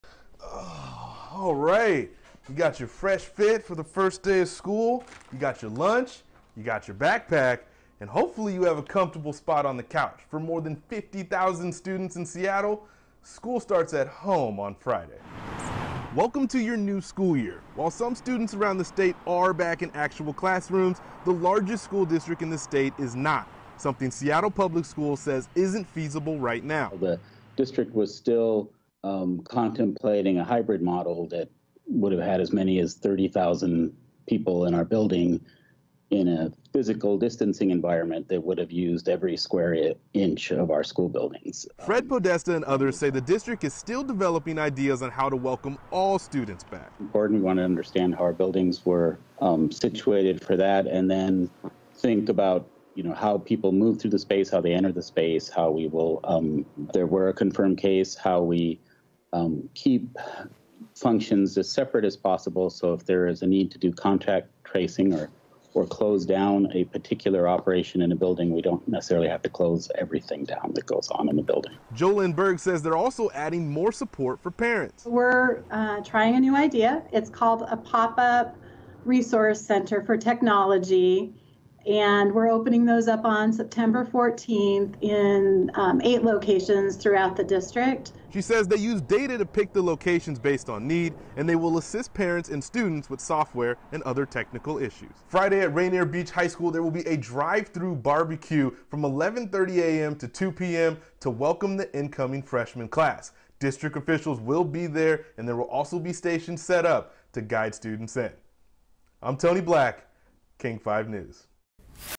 Seattle Public Schools start remote learning Friday
First-12th grade students will start remote classes on Sept. 4 and preschool and kindergarten students will start Sept. 8. Technology Resource Centers have opened.